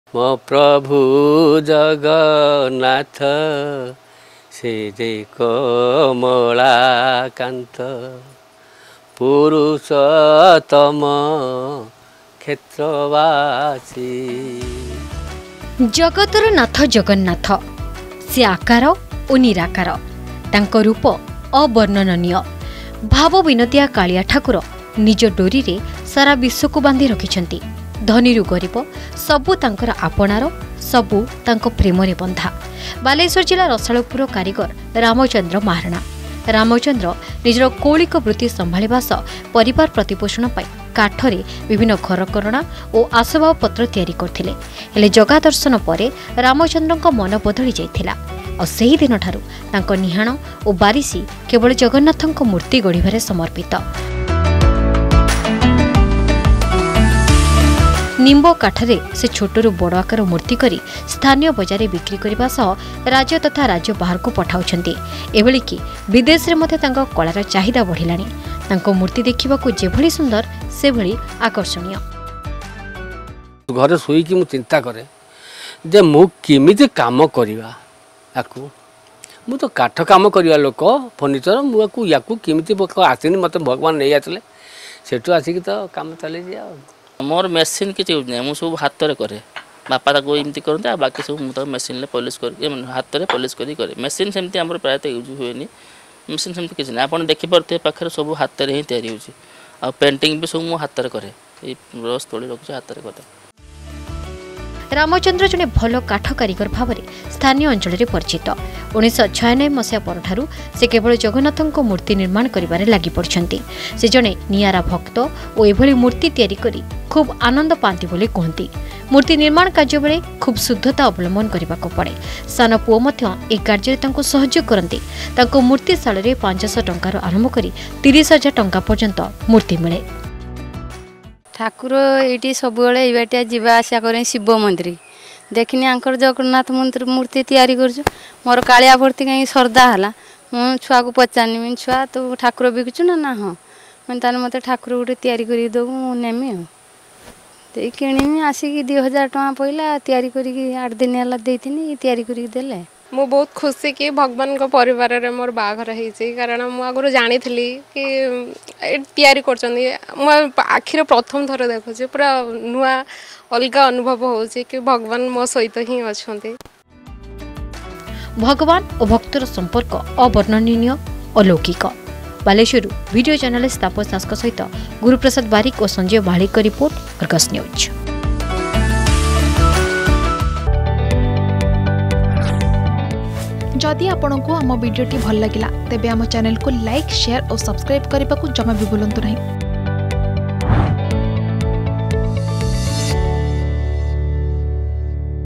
ବାଲେଶ୍ଵର ଜିଲ୍ଲା ରସାଳପୁରର କାରିଗର ରାମଚନ୍ଦ୍ର ମହାରଣା || Jagannatha Karigara
ବାଲେଶ୍ଵର ଜିଲ୍ଲା ରସାଳପୁରର କାରିଗର ରାମଚନ୍ଦ୍ର ମହାରଣା । ରାମଚନ୍ଦ୍ର ନିଜର କୌଳିକ ବୃତ୍ତି ସମ୍ଭାଳିବା ସହ ପରିବାର ପ୍ରତିପୋଷଣ ପାଇଁ କାଠରେ ବିଭିନ୍ନ ଘରକରଣା ଓ ଆସବାବପତ୍ର ତିଆରି କରୁଥିଲେ। ହେଲେ ଜଗା ଦର୍ଶନ ପରେ ରାମଚନ୍ଦ୍ରଙ୍କ ମନ ବଦଳି ଯାଇଥିଲା ।